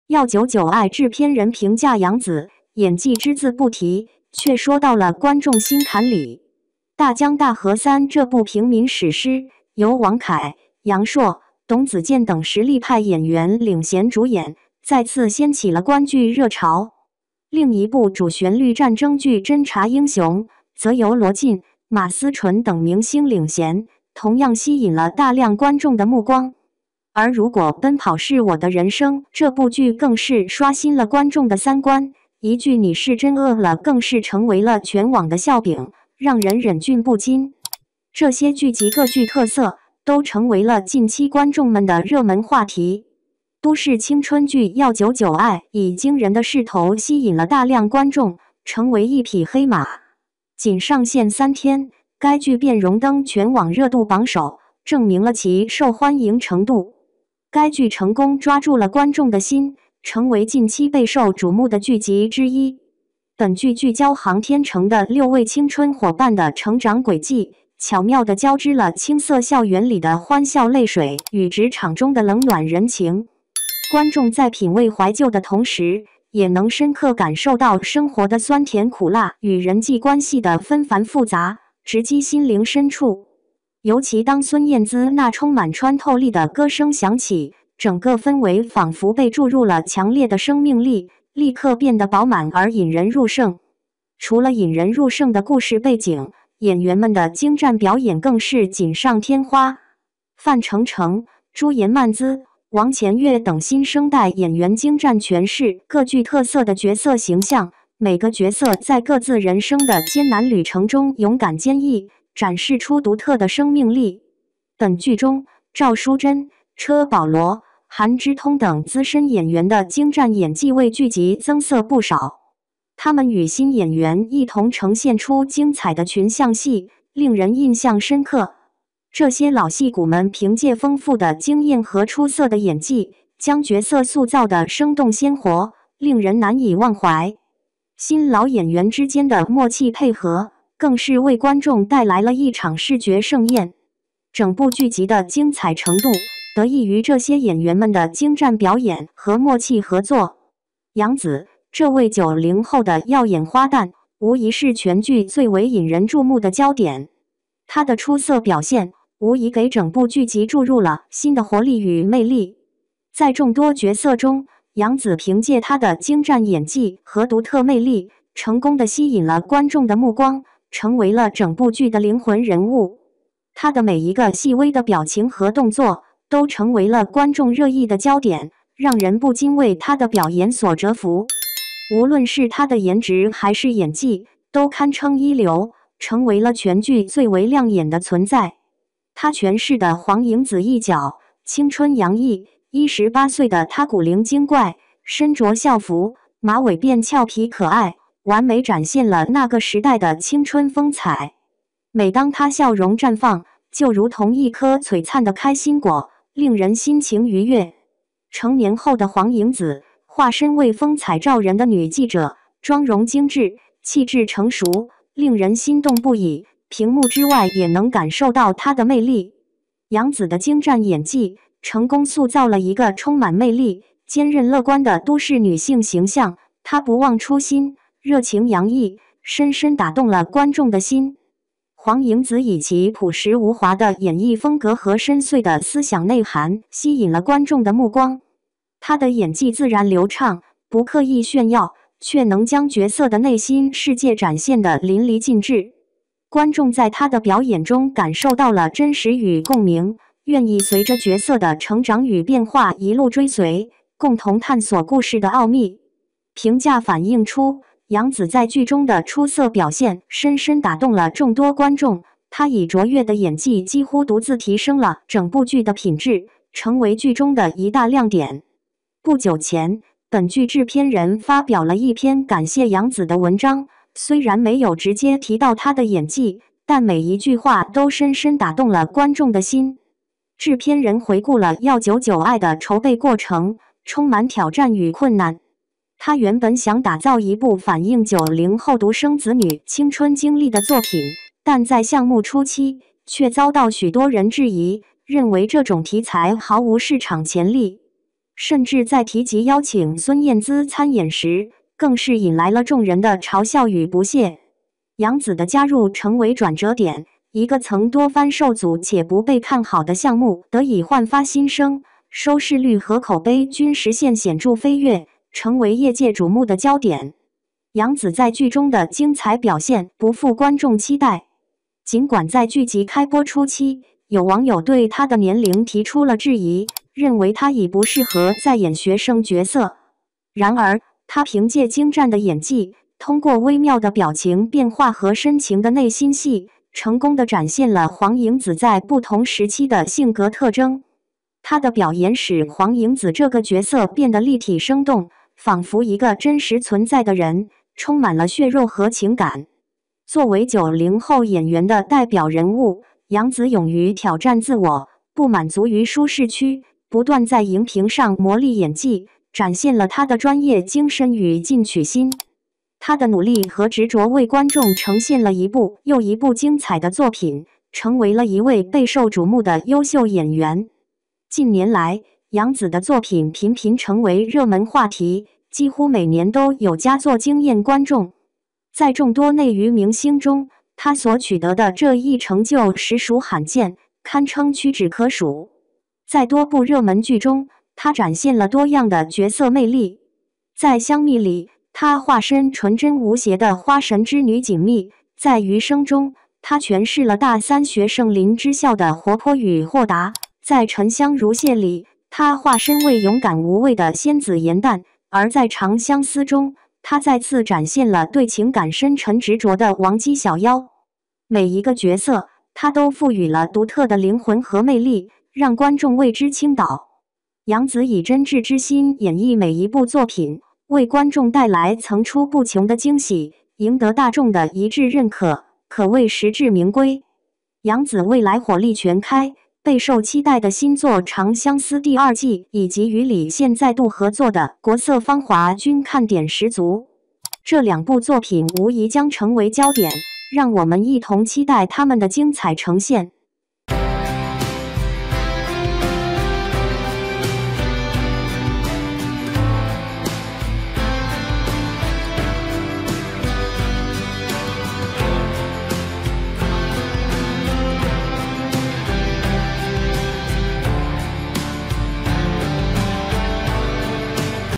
《要久久爱》制片人评价杨紫，演技只字不提，却说到了观众心坎里。
《要久久爱》制片人评价杨紫，演技只字不提，却说到了观众心坎里。| 八八娱乐呀